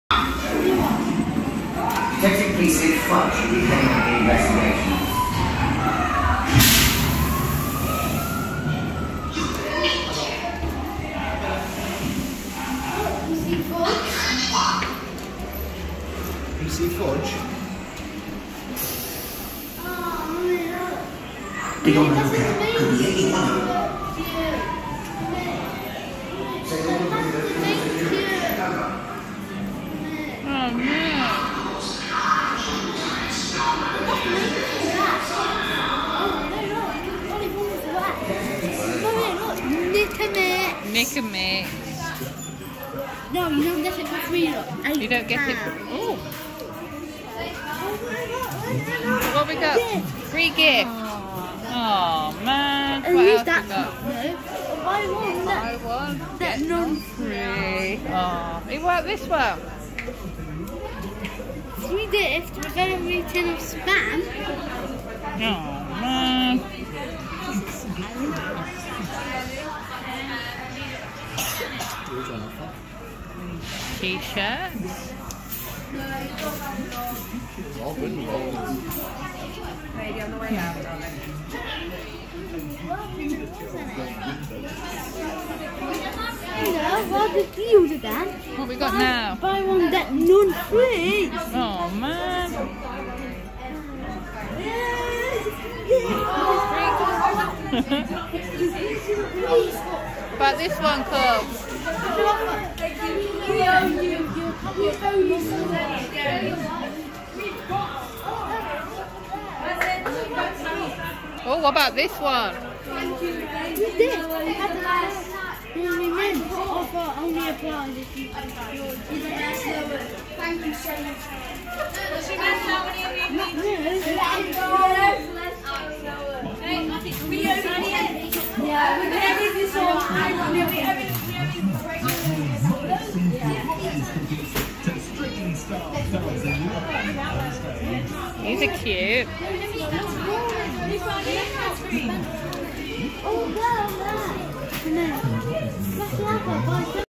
GANGSTA GRANNY - Raj's shop | Alton Towers
Walk around Raj's shop at the end of the GANGSTA GRANNY the ride in the world of David Walliams at Alton Towers.